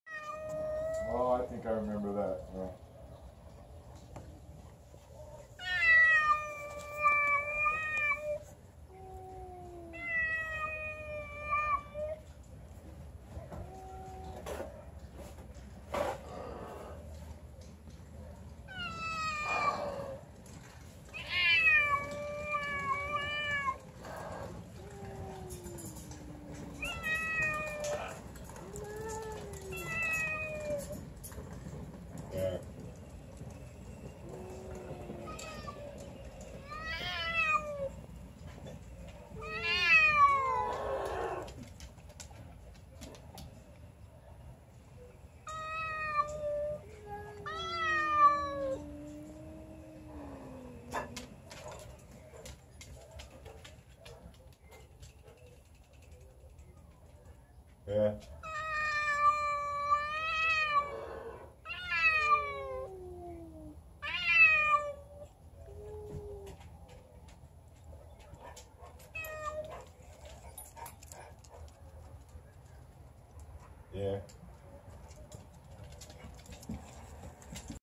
Kiki (black, female) VS. Coconut (blond, male)
There's an epic battle on the front pirch! Or, perhaps a mating ritual, or an argument about food or territory? I do not know but it's been going on for years. MeowiageCounseling? Keeheeheehee! Yes, THIS is normal here. Meow.